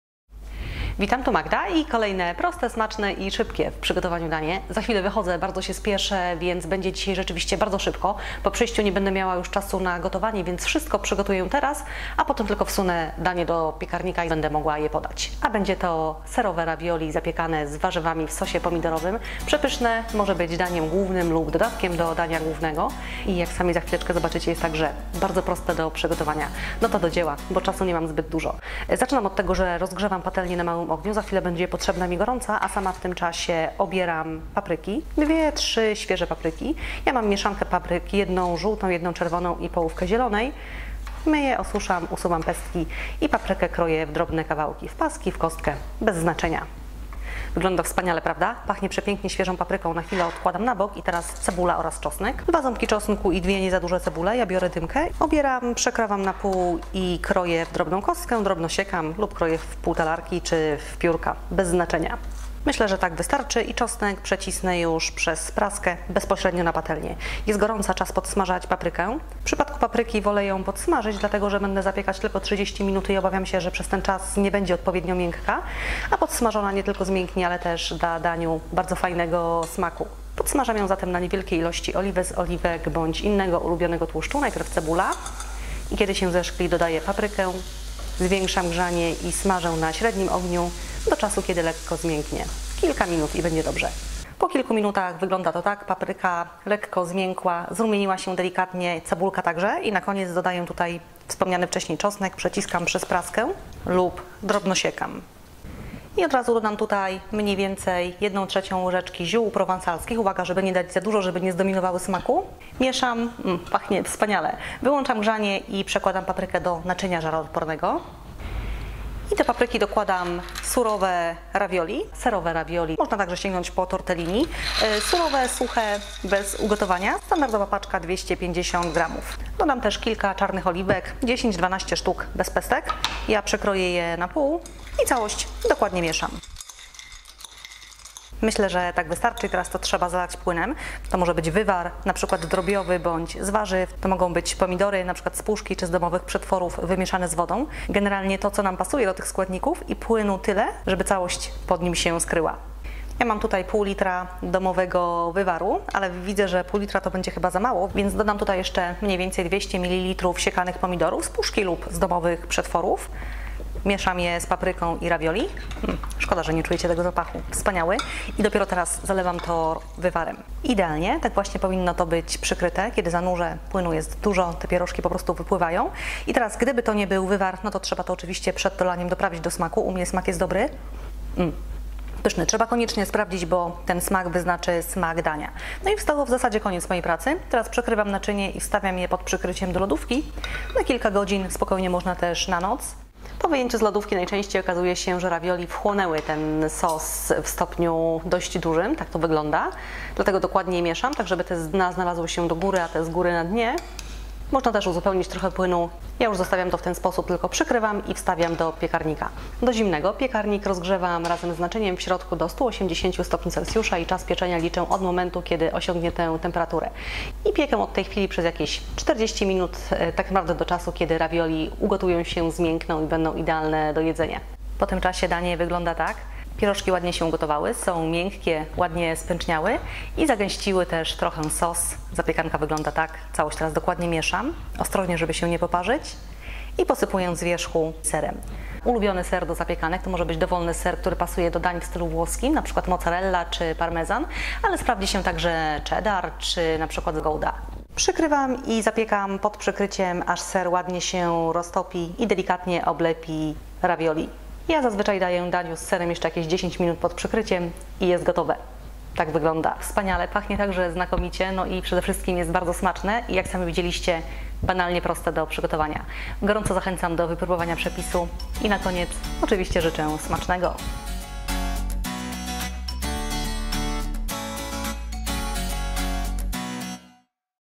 Zapiekane ravioli serowe z papryką :: Skutecznie.Tv [HD]
Miękkie, serowe pierożki, najpierw duszone z warzywami, a potem zapieczone pod serem… Całość cały czas dzieje się pod przykryciem w piekarniku, więc wszystkie smaki powoli się przenikają, dając finalnie pyszne zapiekane ravioli z papryką. Warzywa miękkie, ale nie rozgotowane, pierożki przyjemne do jedzenia, bo gotowały się nie w wodzie, ale w pełnym smaku wywarze, z którego na końcu powstaje wyborny sos. Całość banalnie prosta, nakład pracy minimalny, a efekt bardziej niż zadowalający. Smakuje i dzieciom, i dorosłym.

TOP 20